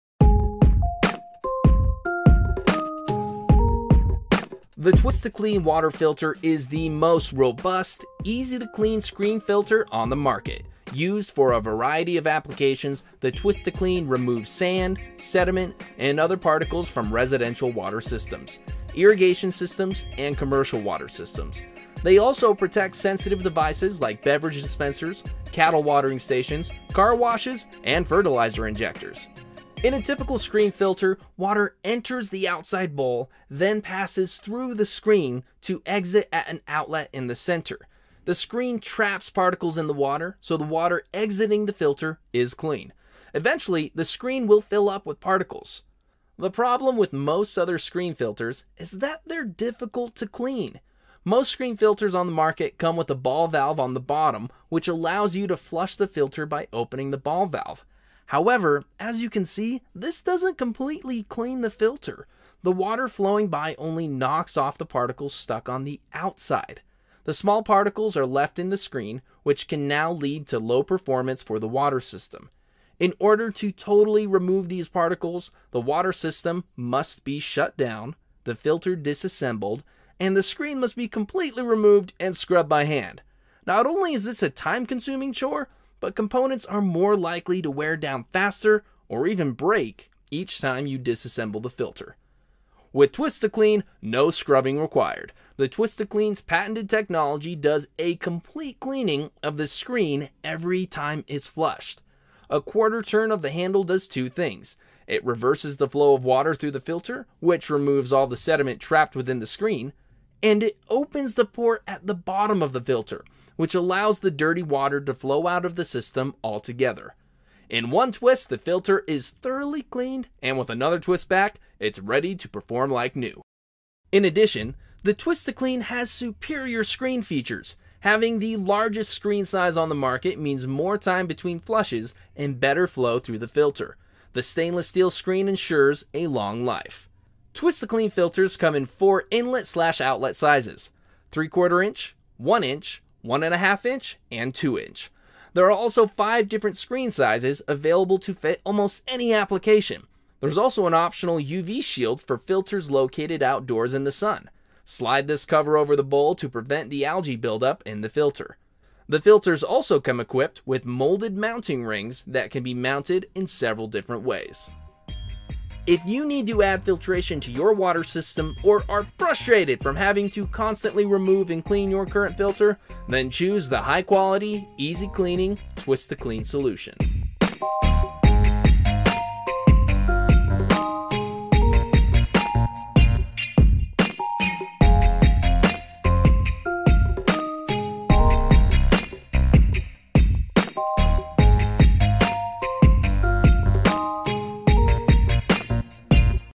【東電研】TwistIIClean 產品及應用 - T2C反洗式雜質過濾器
美國TwistIIClean前置雜質過濾器，不鏽鋼精密濾網高效攔截水中泥沙、顆粒雜質...等汙染物，保障全家人生活用水健康。專利反沖洗功能保障水質安全，並延長濾網使用壽命

TwistIIClean | 東電研淨水事業部